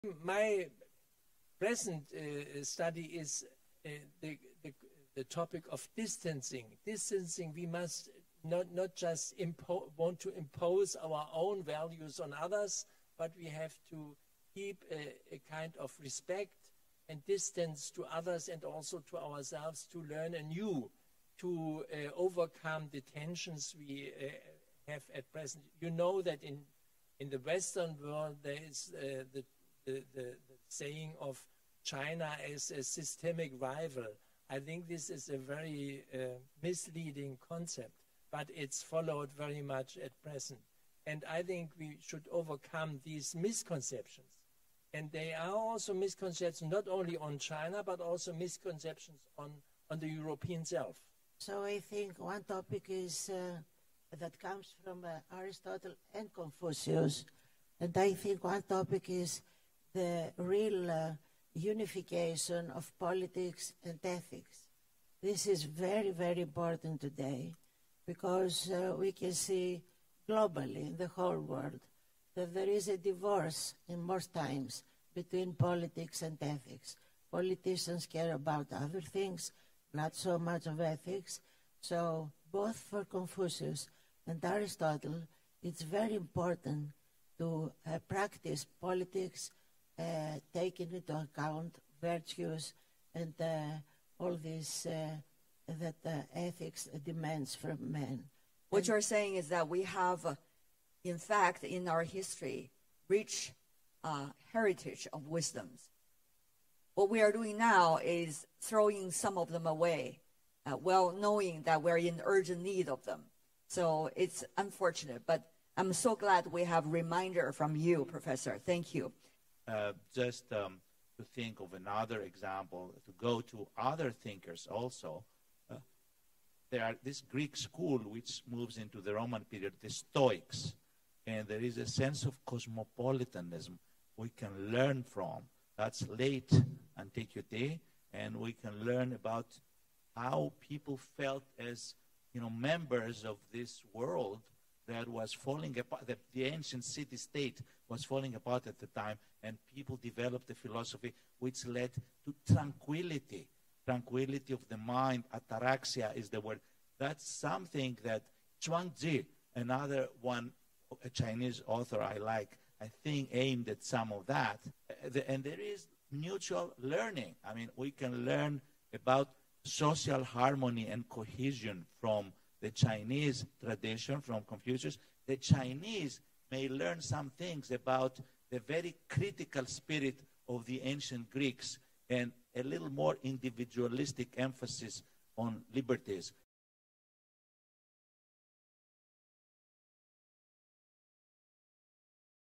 The Ninth Nishan Forum: Mutual learning among civilizations on overcoming global challenges
The Ninth Nishan Forum on World Civilizations is being held in Qufu City, east China's Shandong Province, the hometown of Confucius. CGTN's Tian Wei was invited to host the event "Nishan Interview with Diplomatic Envoys and International Scholars". Six guests from Africa, Europe and Latin America gathered to talk about the exchanges and mutual learning among civilizations and the building of a community with a shared future for mankind. What can ancient wisdom from both the East and West teach us about overcoming modern tensions and misconceptions?  How can we unite politics with ethics? What's the connection between Confucius' philosophy and Aristotle's? For more insights, let's listen to the voices from Nishan!